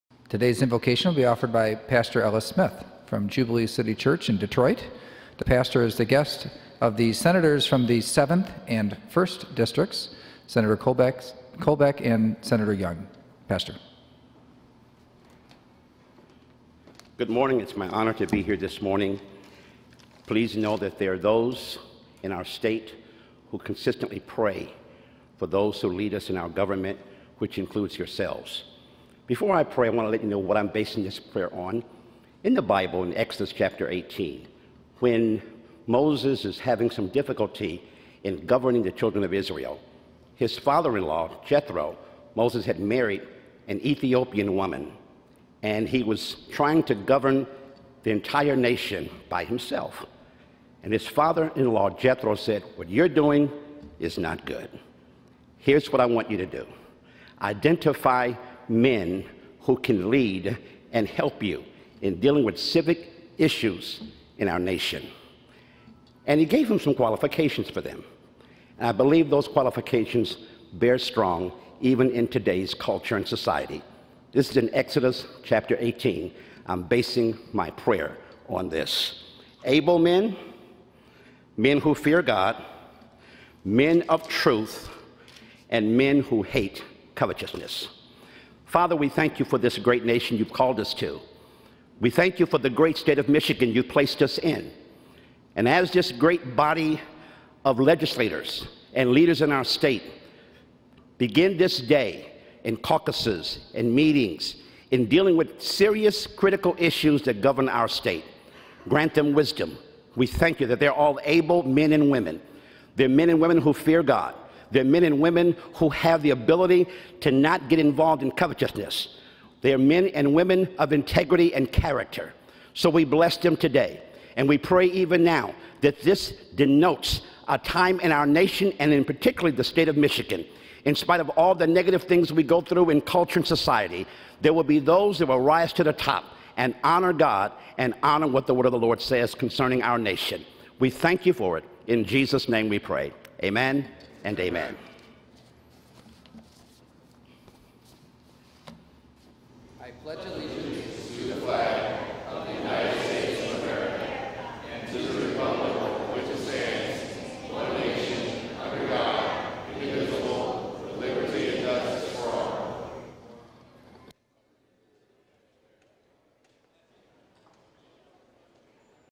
Sen. Colbeck welcomes Pastor Smith to deliver invocation to the Senate
Senator Patrick Colbeck, R-Canton, welcomed Pastor Ellis Smith of Jubilee City Church in Detroit to the Michigan Senate to deliver the invocation at the start of the March 10, 2016 session.